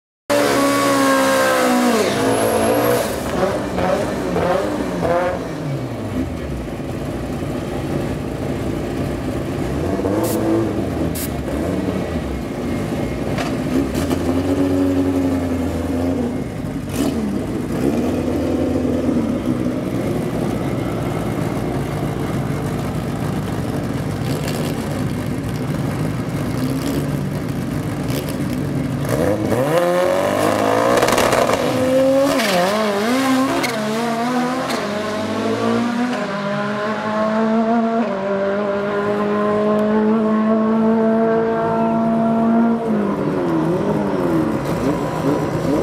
July 2017 QTM Day 1 Run 26
20th July 2017 - Day 1 (Practice Day) of the third Quarter Mile Event (The flames of Fire) part of the 2017 MDRA championship.

Tomorrow races continue at 6pm with Qualifying and Dial'ins.